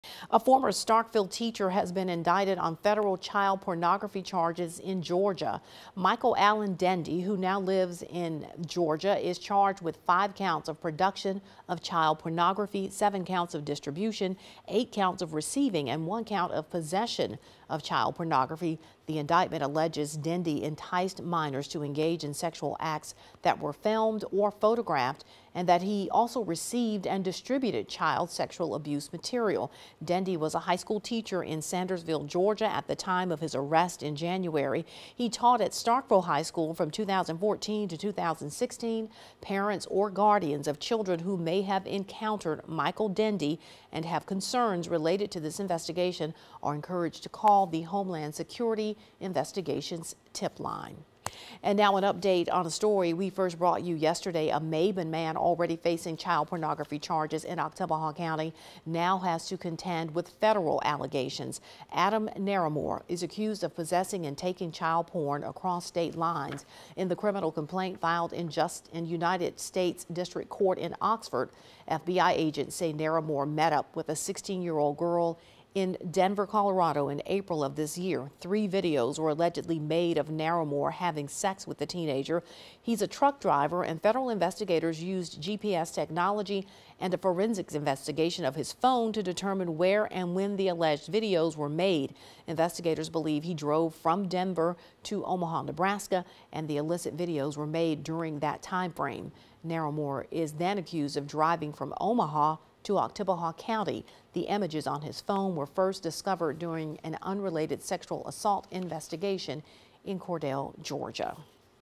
Former Starkville teacher indicted on federal child pornography charges
Michael Allen Dendy who now lives in Milledgeville, Georgia was charged with five counts of production of child pornography, seven counts of distribution, eight counts of receiving, and one count of possession of child pornography.